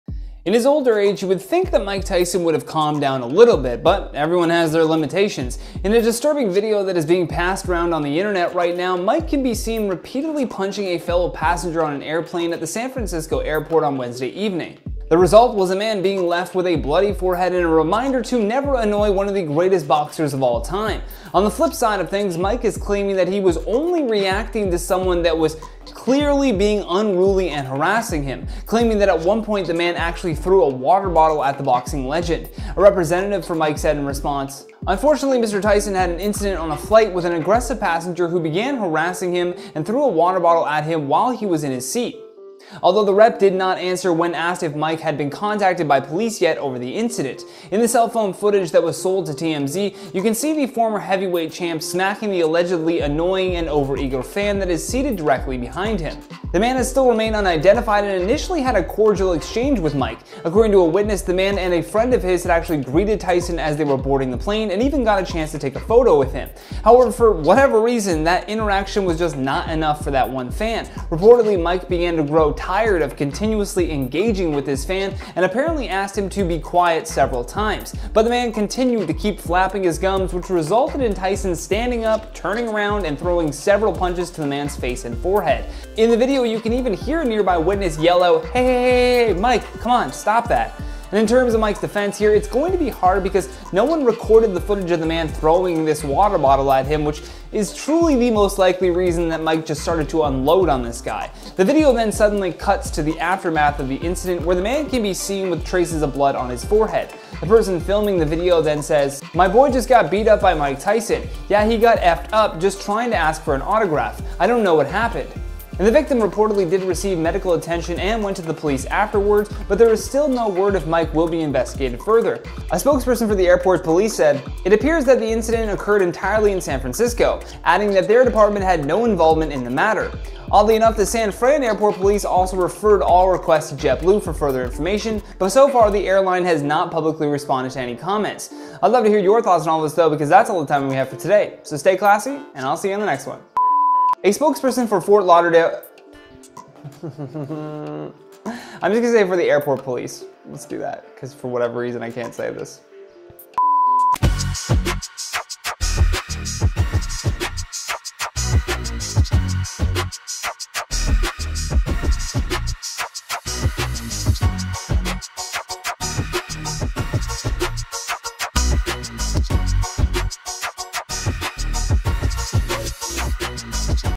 Mike Tyson Caught On Camera Hitting Fan Because Of THIS
In his older age you would think that Mike Tyson would have calmed down a bit, but everyone has their limitations. In a disturbing video that is being passed around on the internet right now, Mike can be seen repeatedly punching a fellow passenger on an airplane at the San Francisco airport on Wednesday evening.

PRODUCER & FILMED BY:
Chris Stiuso